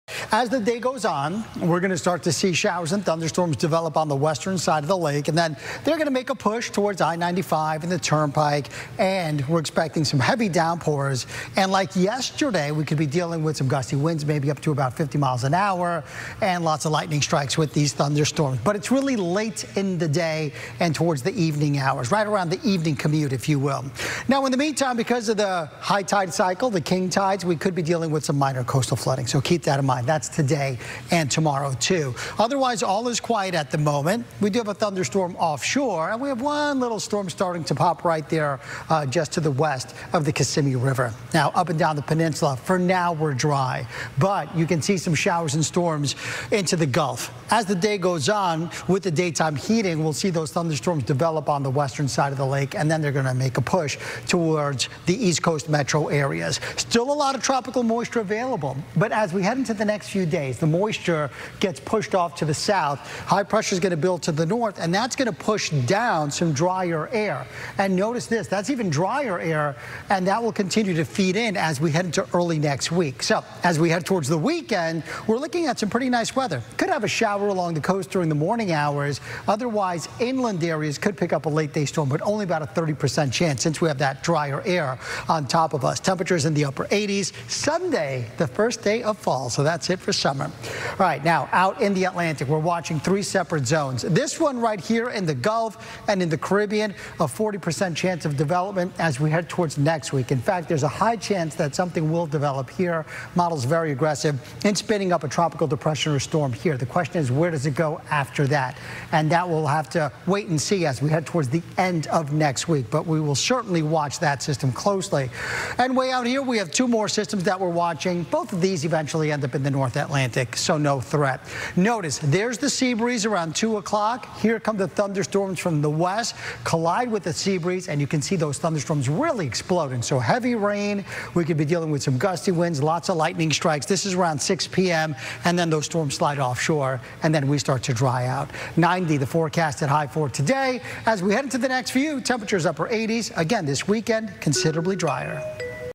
WPTV First Alert Weather Forecast for Afternoon of Thursday, Sept. 19, 2024
Late day storms are in the forecast once again.